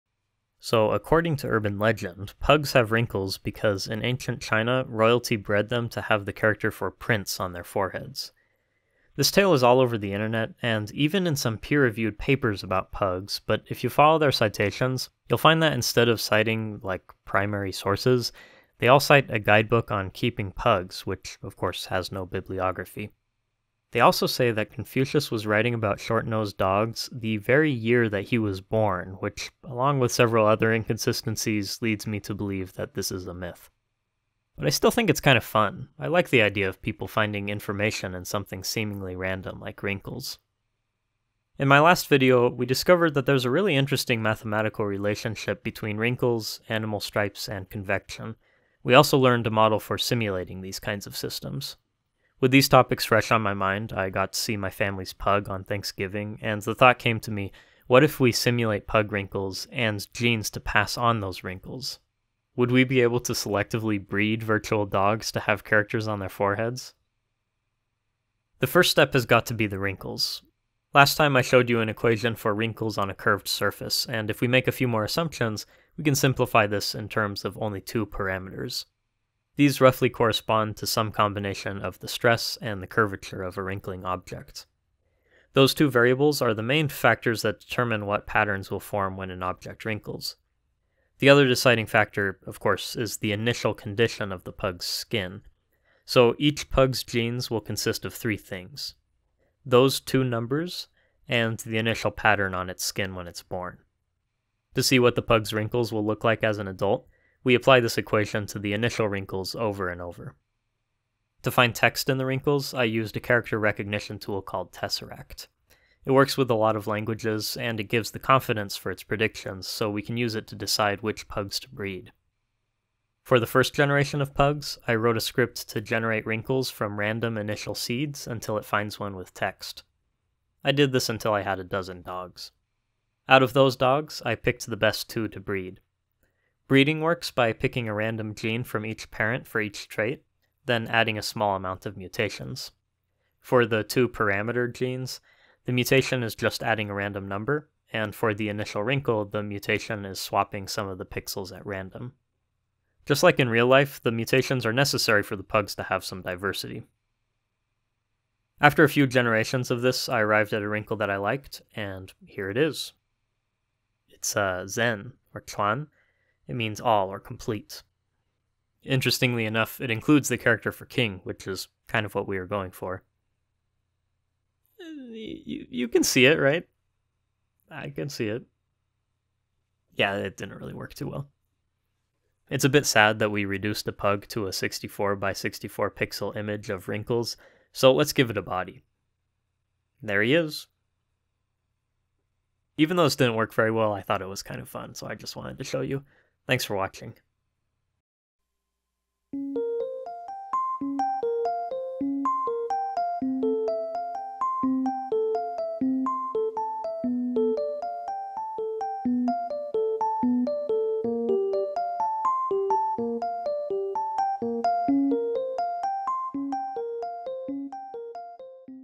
Breeding Virtual Pugs to Have Words in their Wrinkles
Let's use wrinkle simulations to selectively "breed" pugs until they have words on their foreheads!  We'll implement the Turing-Swift-Hohenberg equation in python and see how it looks.

0:00 An Urban Legend
0:40 Getting it to Work
2:37 The Results

Thank you to Caleb Birtwistle for captioning!